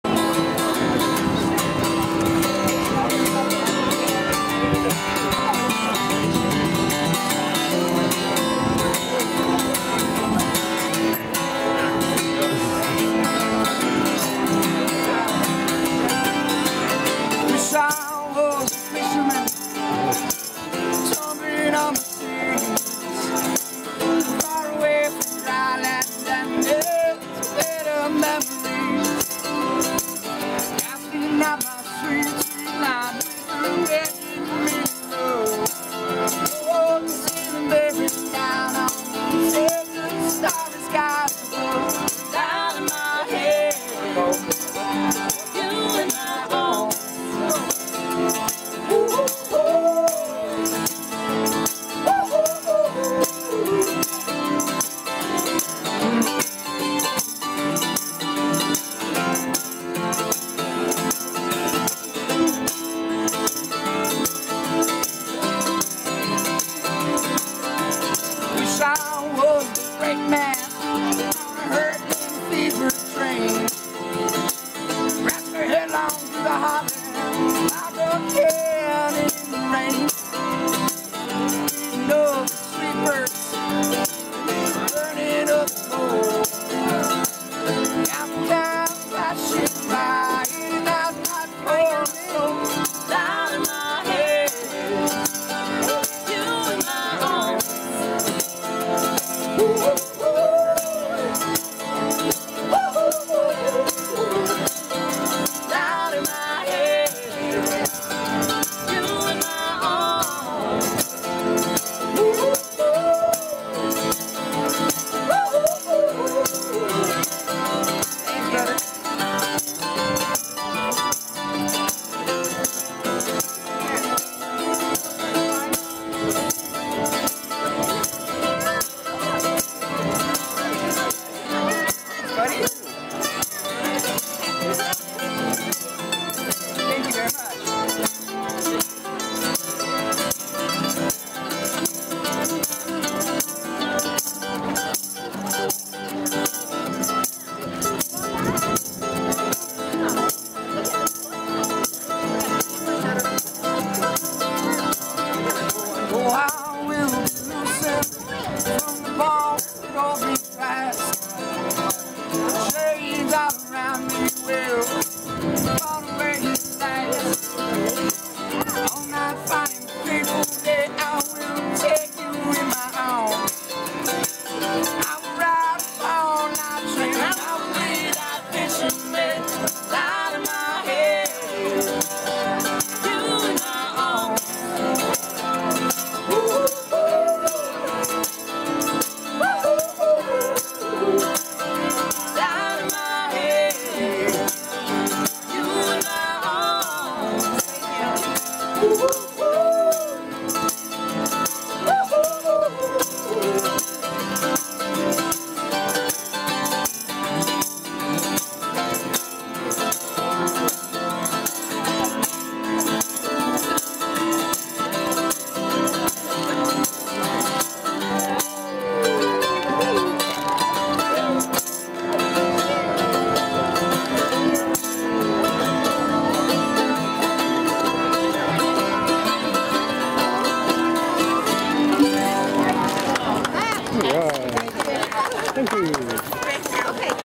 Tiger Moon - Quidi Vidi Lake - St. John's, NL 8-5-15 part 8/9
Tiger Moon performs a free concert at the 197th Royal St. John's Regatta in Newfoundland. More info tigermoonmusic.com twitter @tweettigermoon